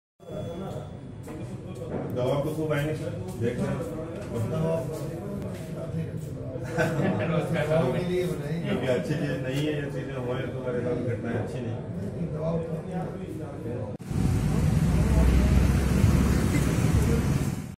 बूंदी में क्यों दी प्रशासनिक अधिकारी ने धमकी
This Channel is News and Entertainment Channel... and given knowledge to people